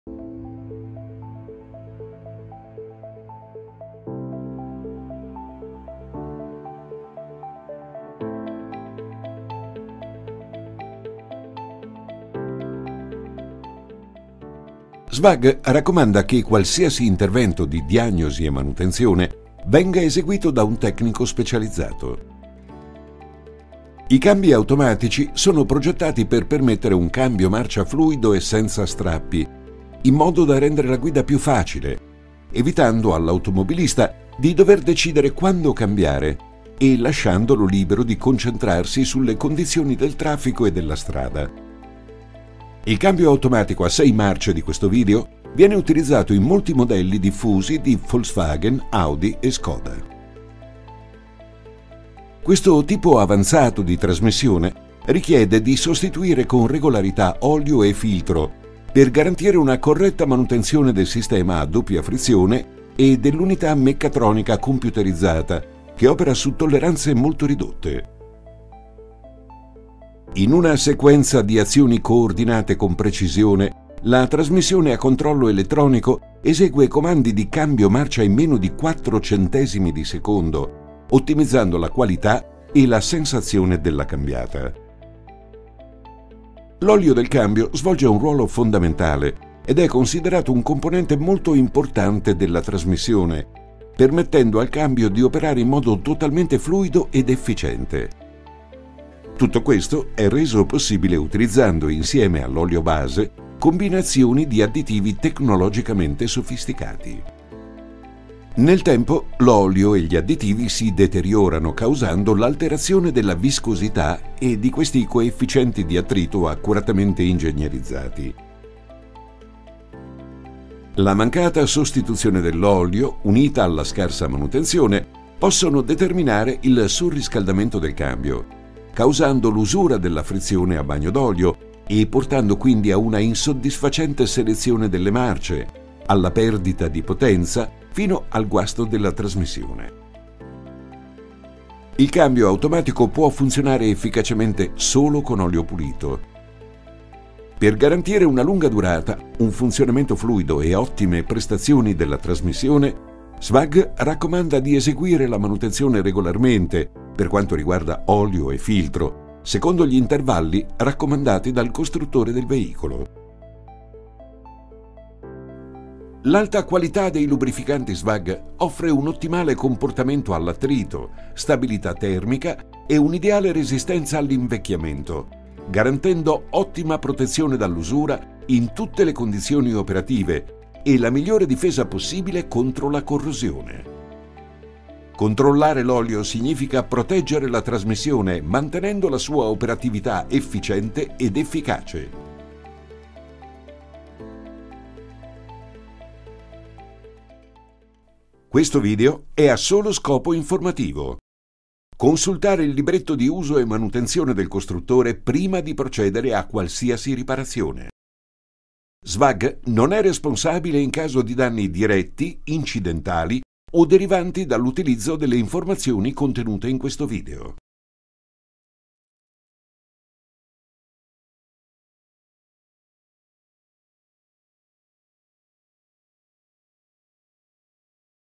[ITA] Sostituzione olio e filtro cambio automatico DSG
In questo video esaminiamo la funzione dell’olio trasmissione che mantiene in funzione la trasmissione stessa e il filtro trasmissione che dovrebbe essere sostituito insieme all'olio durante la manutenzione ordinaria.